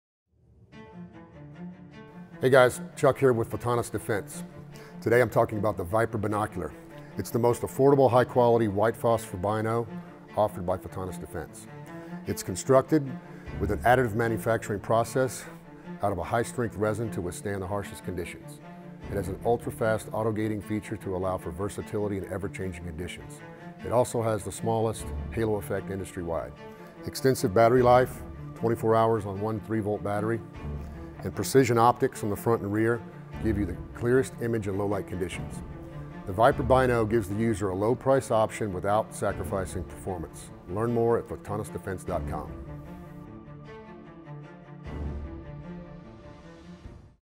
Photonis Defense Vyper 18mm Ruggedized Binocular Night Vision Goggle.
The Photonis Defense Vyper is a lower cost 18mm binocular night vision goggle using two Photonis ECHO or 4G Photonis image intensifier tubes, in a ruggedized resin housing.